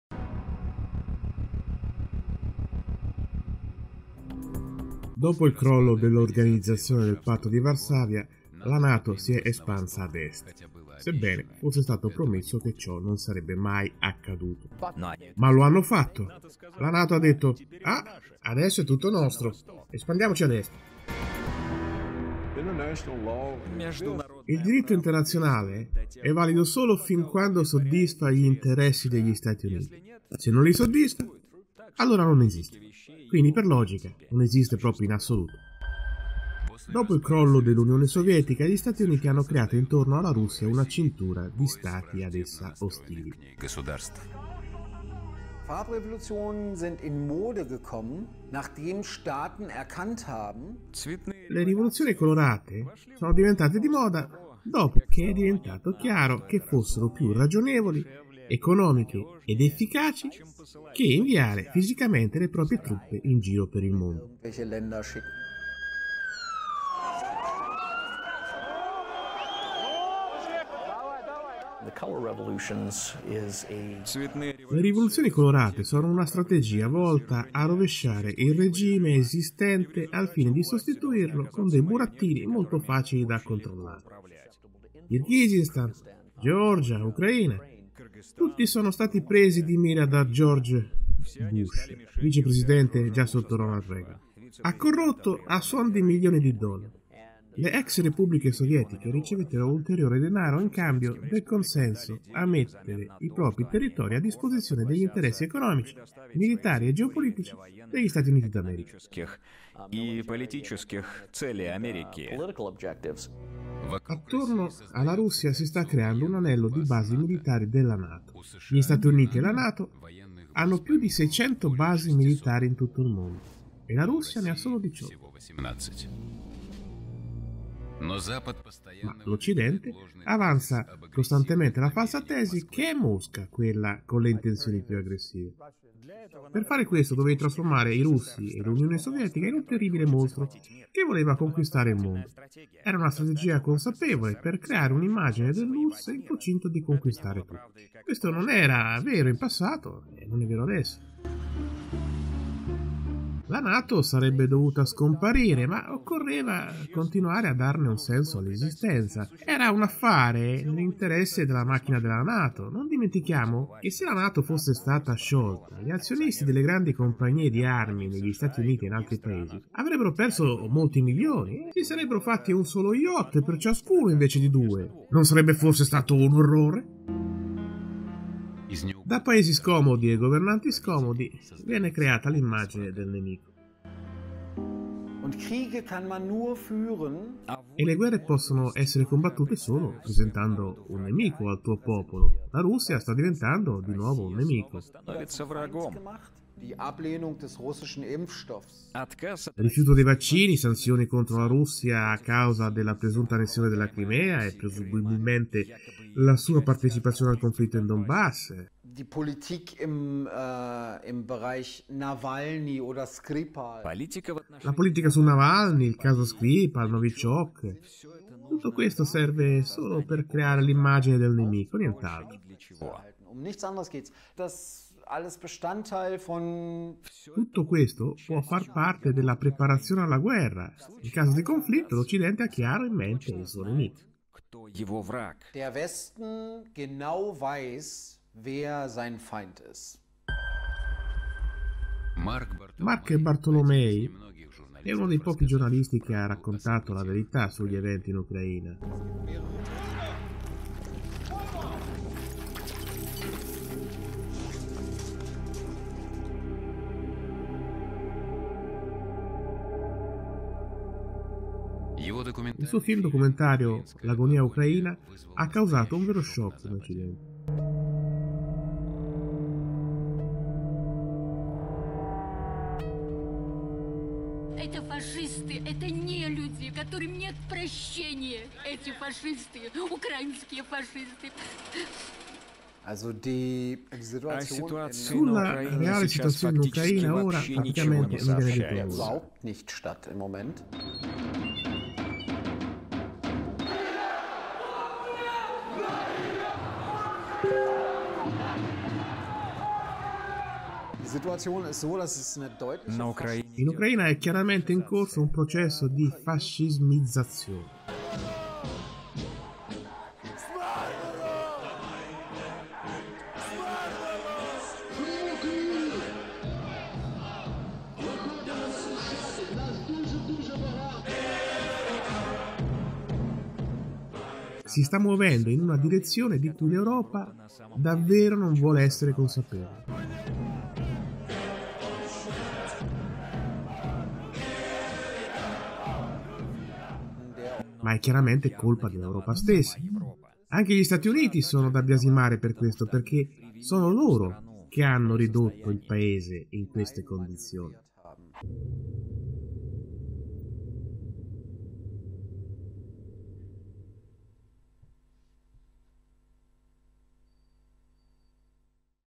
DEMOCRATISMO - sesta puntata
Qui si parla di Proxy war e Proxy rivoluzioni. Come dice il giornalista di inchiesta tedesco Marc Bartolomei, nell’epoca nucleare “Le 'rivoluzioni colorate' sono diventate di moda dopo che è divenuto chiaro che fossero più ragionevoli, economiche ed efficaci che inviare fisicamente le proprie truppe in giro per il mondo”.
Segui il canale Telegram per altri approfondimenti